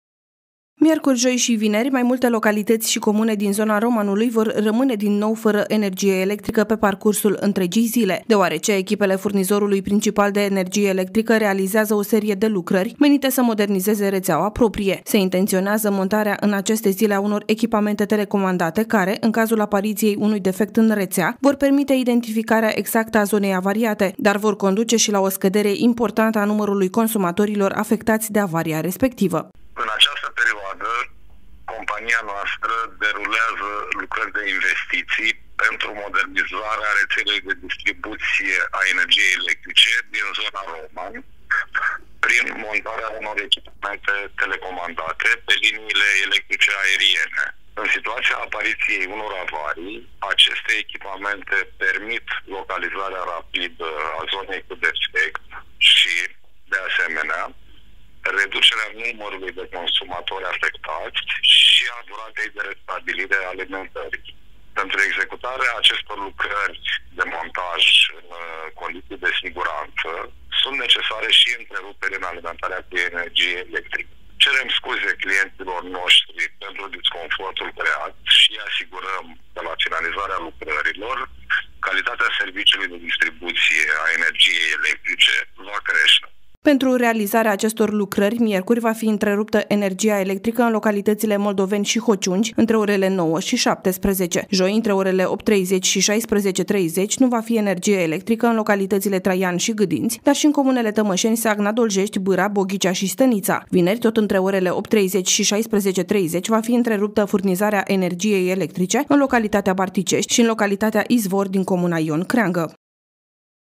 Fără energie electrică, pentru modernizări ale rețelei
Miercuri, joi și vineri, mai multe localități și comune din zona Romanului vor rămâne din nou fără energie electrică pe parcursul întregii zile, deoarece echipele furnizorului principal de energie electrică realizează o serie de lucrări menite să modernizeze rețeaua proprie. Se intenționează montarea în aceste zile a unor echipamente telecomandate care, în cazul apariției unui defect în rețea, vor permite identificarea exactă a zonei avariate, dar vor conduce și la o scădere importantă a numărului consumatorilor afectați de avaria respectivă.